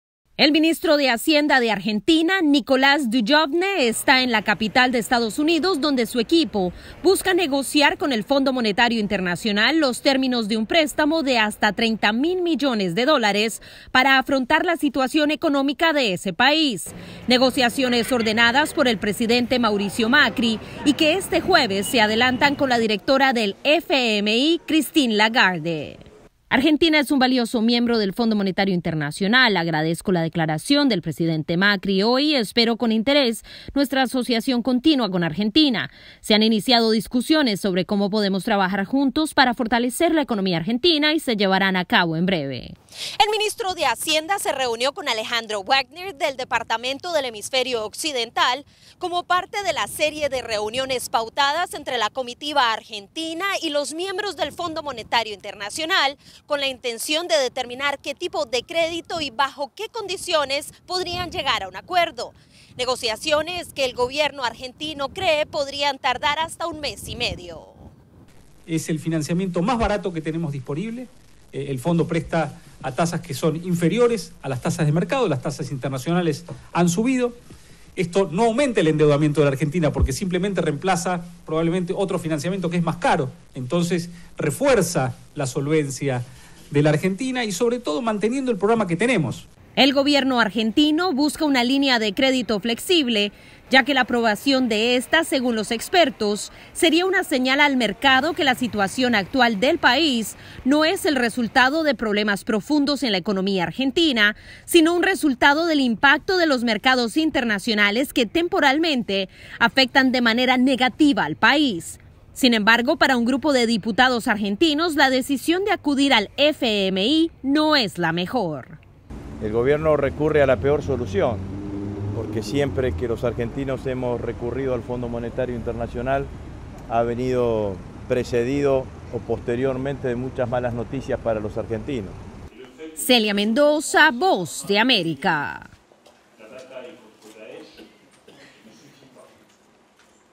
FMI negocia préstamo con Argentina
La capital de los Estados Unidos es el lugar de encuentro entre la comitiva del gobierno Argentino y el equipo del Fondo Monetario Internacional que se reúnen para determinar qué tipo de préstamo y bajo qué condiciones Argentina podría recibir miles de millones de dólares.
Publicado originalmente